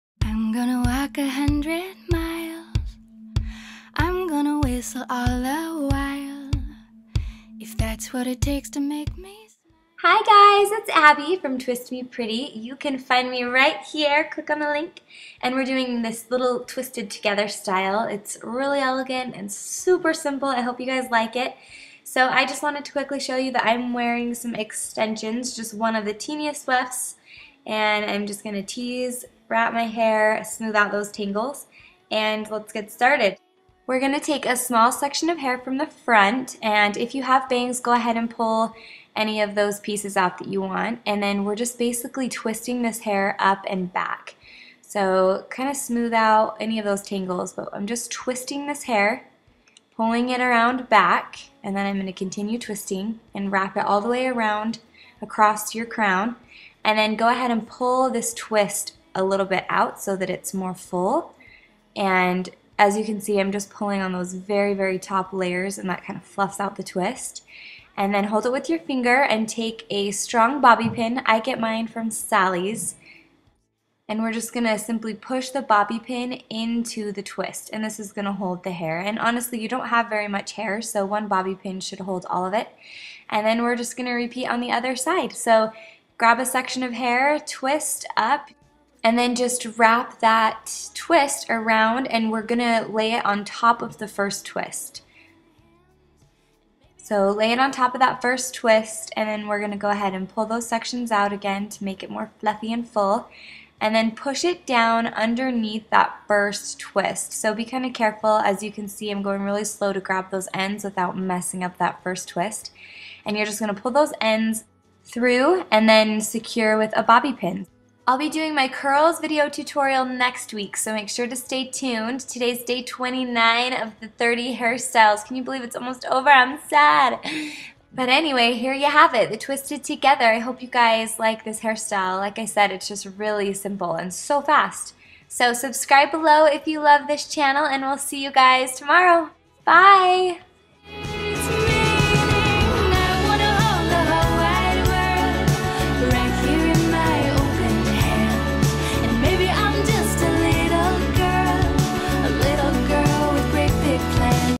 Twisted Together
Come hang out with me on my other social medias!

Day 29. WHA?! I can't believe tomorrow is our last day in the 30|30 challenge. I don't know about you, but I've had such a blast! I am certainly ready though to take a tiny hair break- holy cow that was intense. Remember, I'll still be blogging and I promise to come out with a new hairstyle/beauty video at least once a week. Promise me you'll still visit... Okay good.

Today's hairstyle is one of my favorites. I love half up, half down styles, especially ones that are this easy. This style literally takes less than four minutes from start to finish and it works great with left over curls. So yes, I'm a huge fan. What do you think? Can't wait to see how you style it!